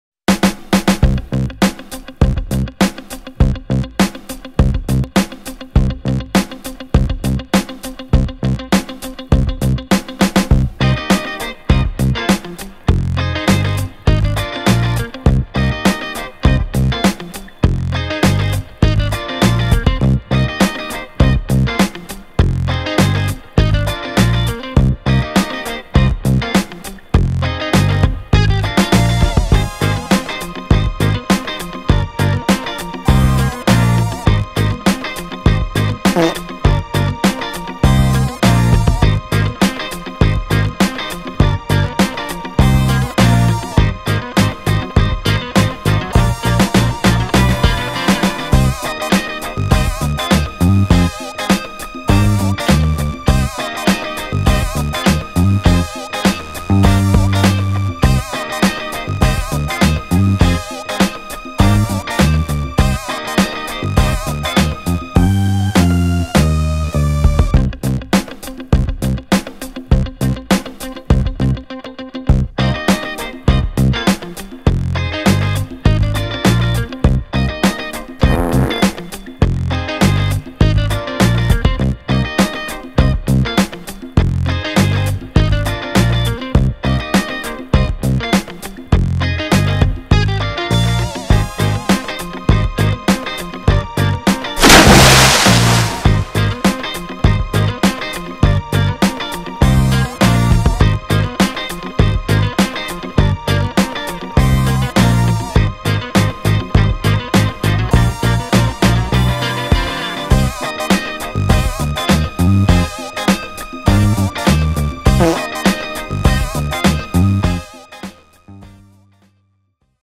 Cosas que una chica piensa al ir por primera vez al baño de su novio
Si esta no es tu peor pesadilla, no sé cuál sea.